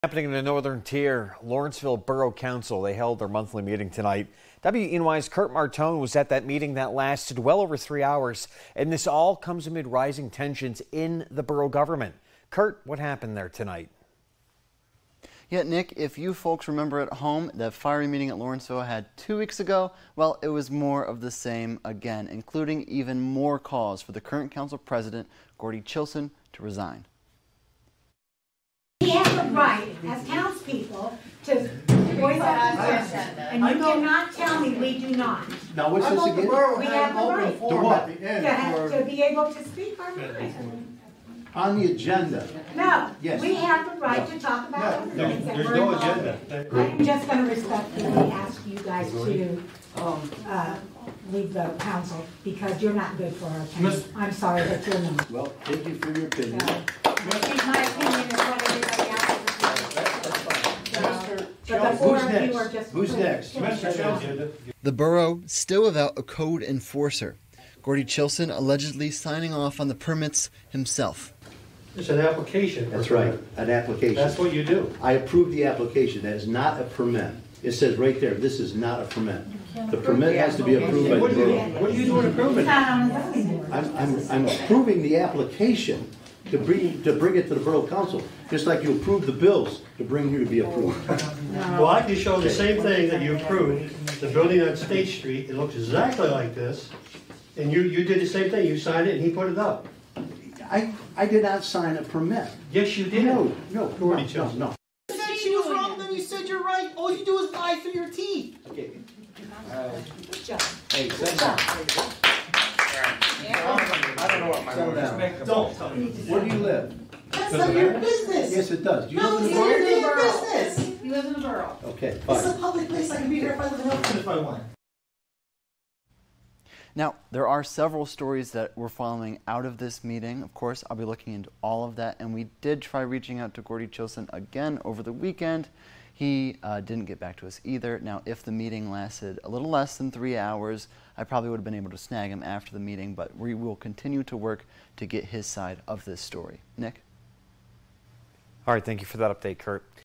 Lawrenceville Council President at center of calls to resign again
On Monday's monthly borough council meeting, which lasted over three hours, in the Lawrenceville Borough, community residents called on Gordy Chilson to resign yet again.